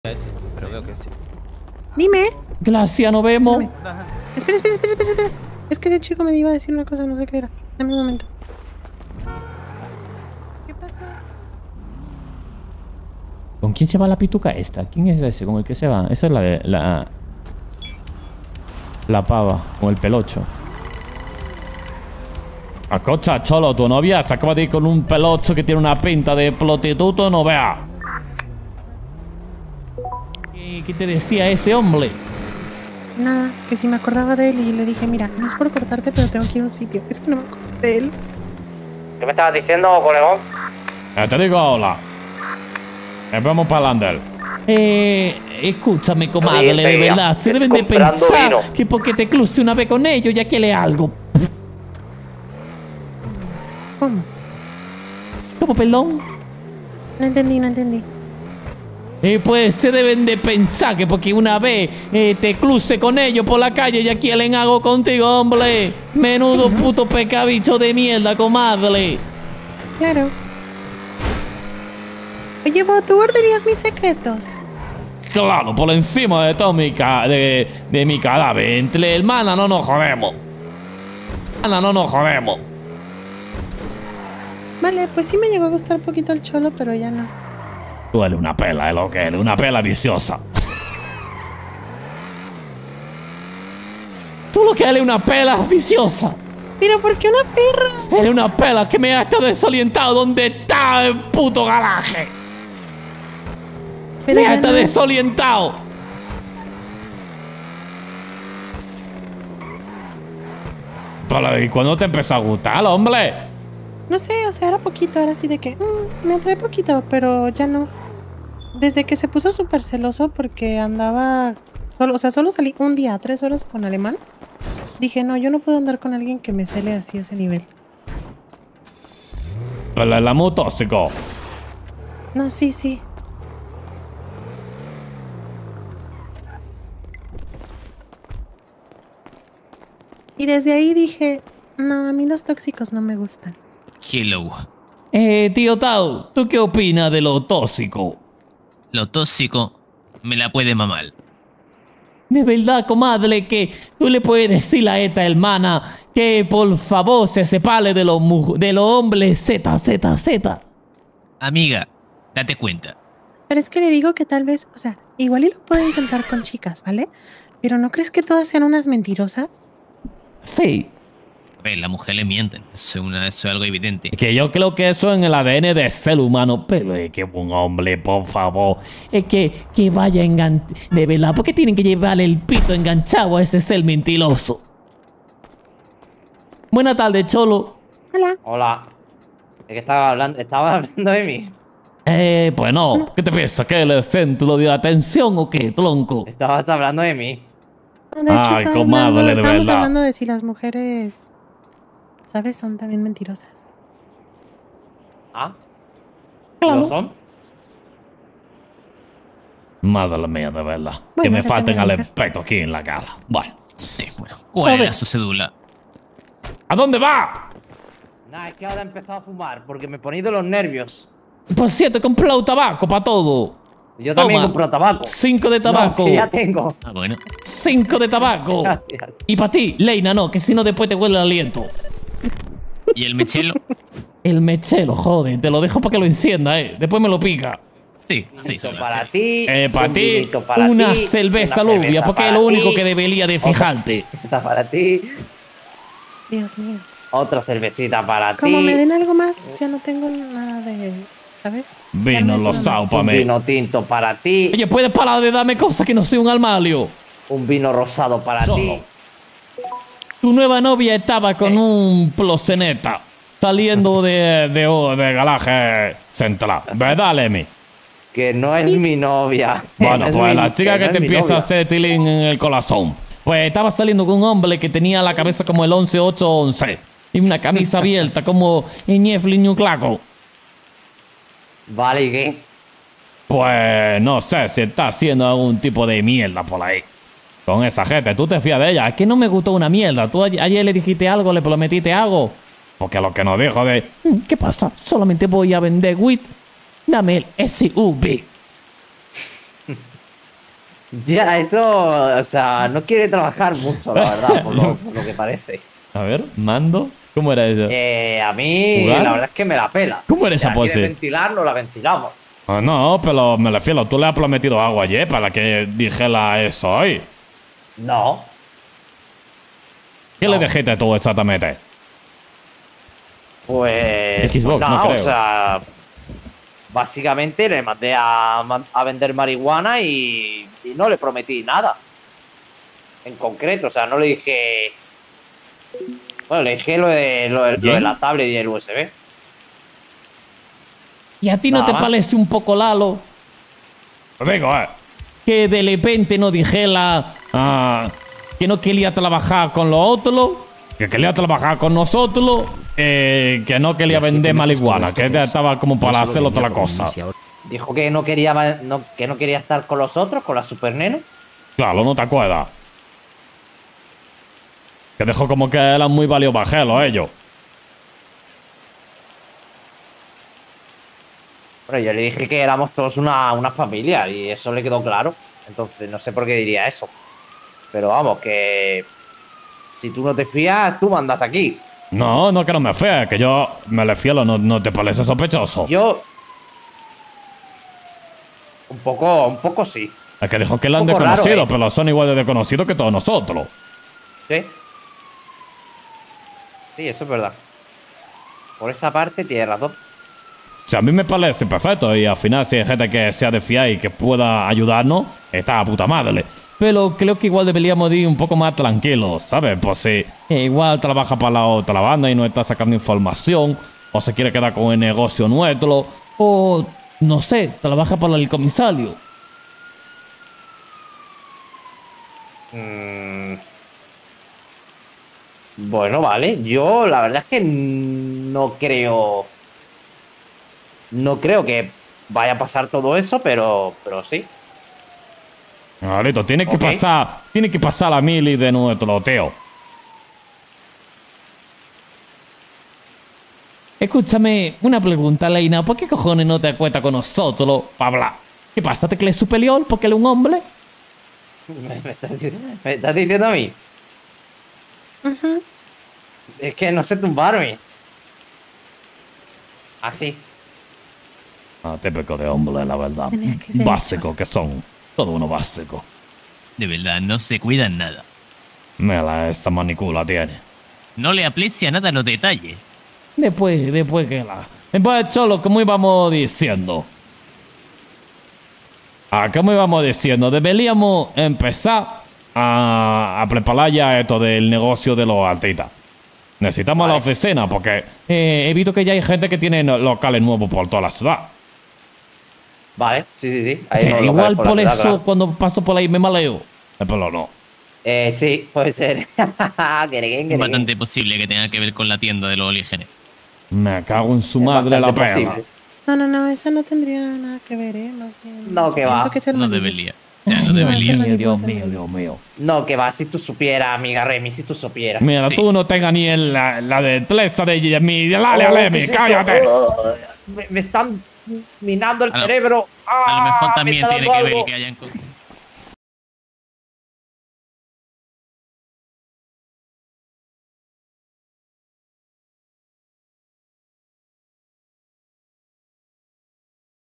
REMI CONFIESA a BO QUE ESTABA ENAMORADA del CHOLO, CHOLO TIENE NUEVA NOVIA? SpainRp
💜CANALES💜
desst3 [ Freddy o Bo]
deqiuv [El Abuelo]
imantado [ Isidoro]
axozer  [Luisito]
nexxuz  [Harry]